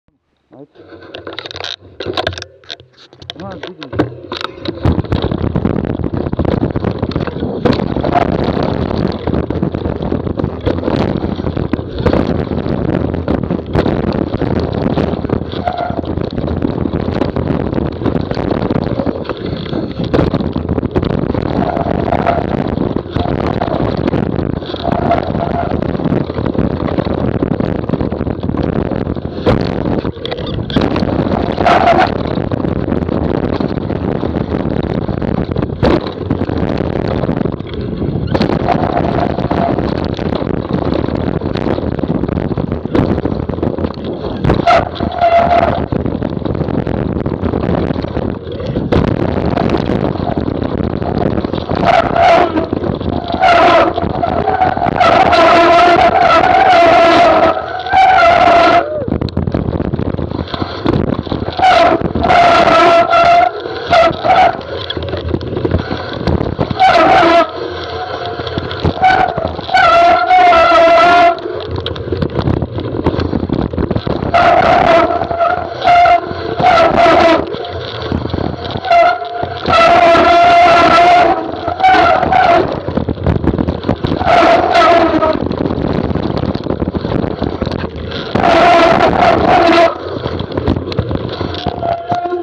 2 Alpes bike park 2015. - Sapins gornji dio staze
Sapins gornji dio staze, snima Franjo Hukman (Norco Aurum), ispred Ivo Jarić (Transition Bandit 29)